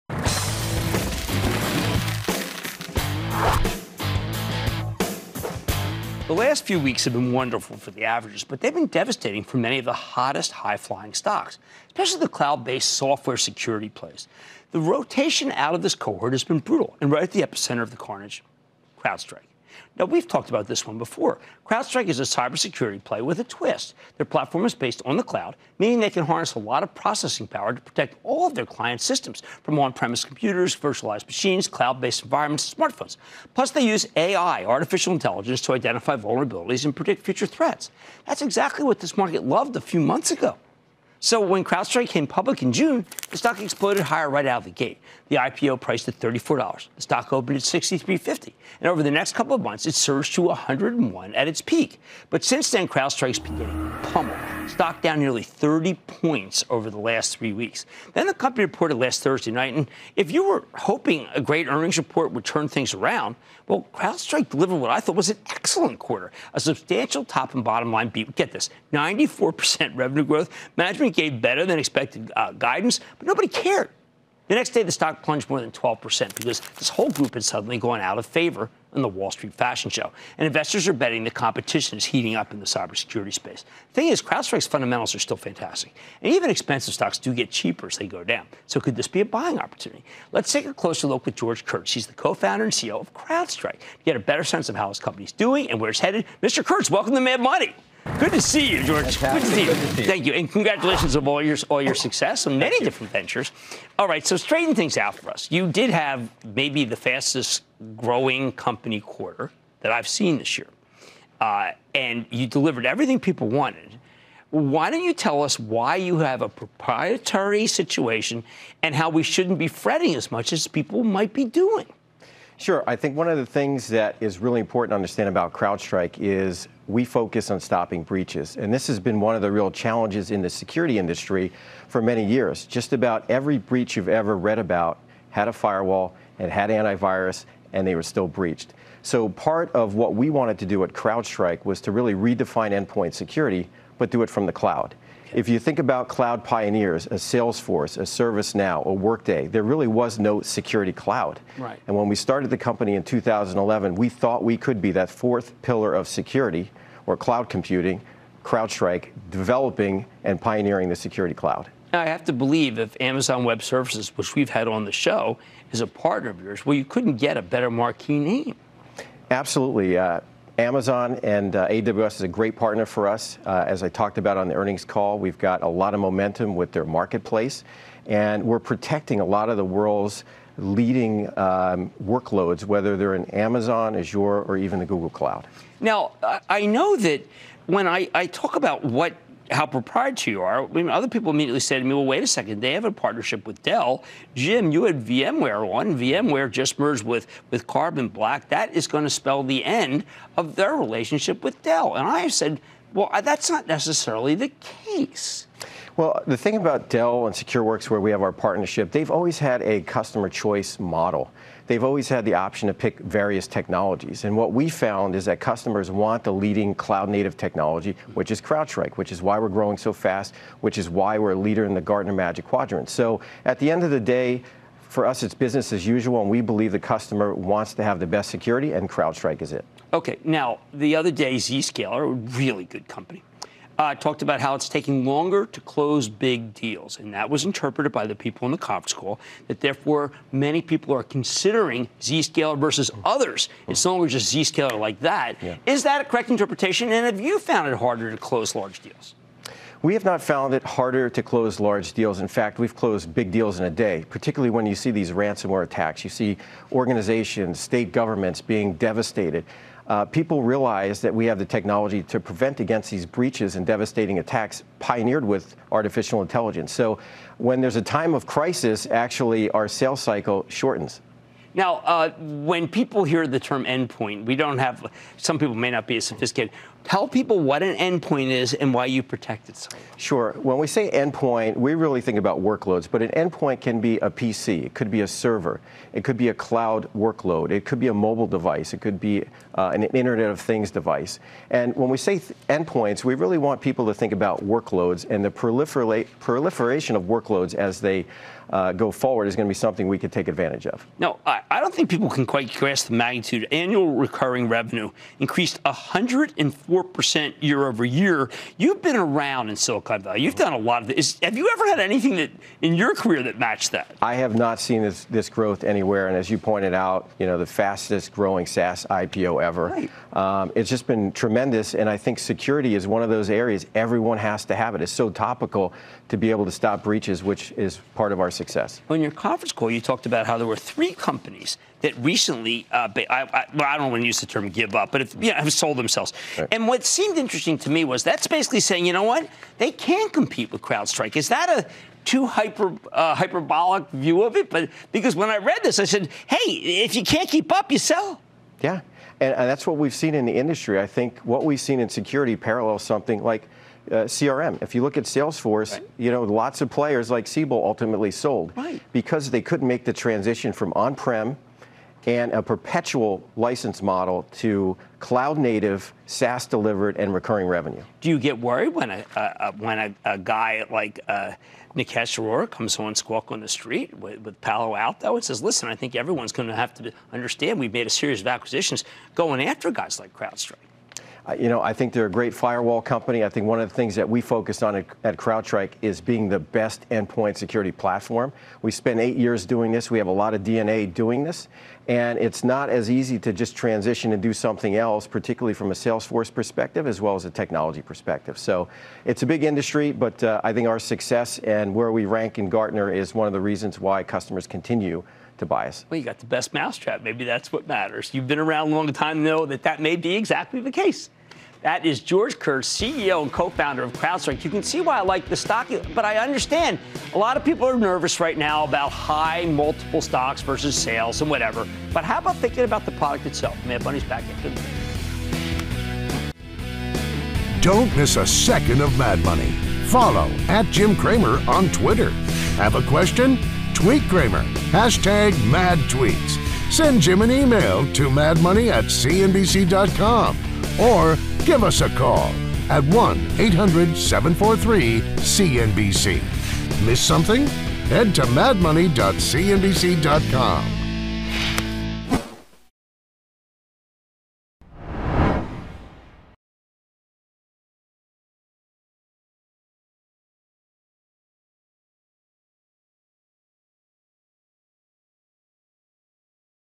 Crowdstrike CEO says upstart competitors don't worry him because "we have a lot of DNA" in cybersecu
Crowdstrike CEO George Kurtz discusses his company's prospects with CNBC's Jim Cramer